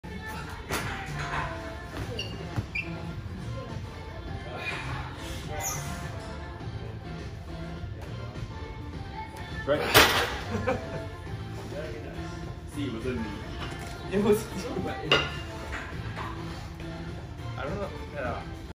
Bench Miss 118 kg
Bench Miss on 1RM test day. 118 kg.